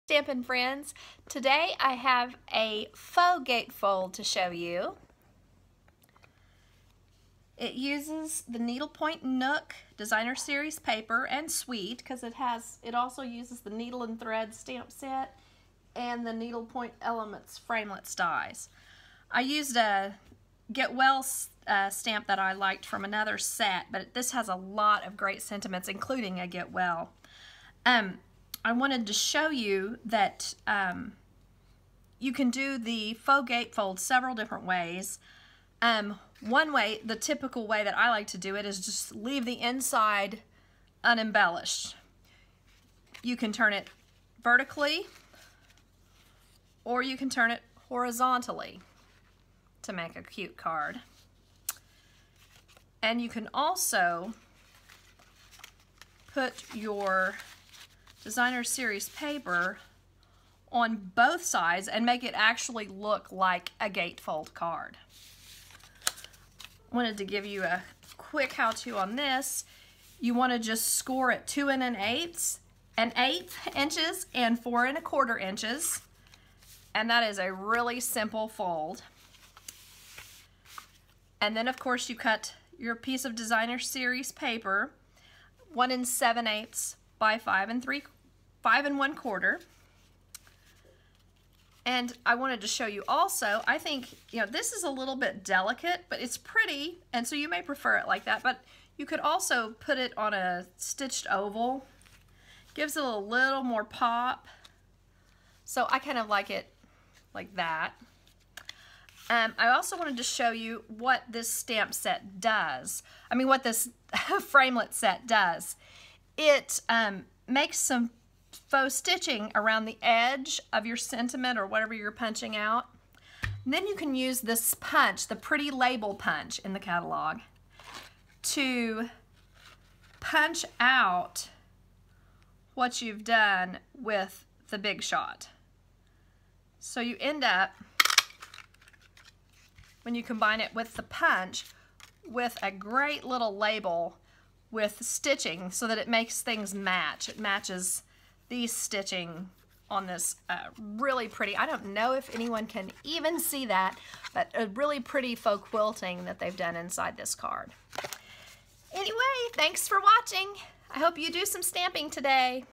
Faux Gatefold Fun Fold Needlepoint Nook Stampin' Up! Card
A Quick as a Bunny video to show you some Faux Gatefold tips using the Needlepoint Nook Suite from the Stampin' Up! 2019 Occasions catalog.  This suite makes it so easy to create collage/cluster style embellishments with flowers and foliage.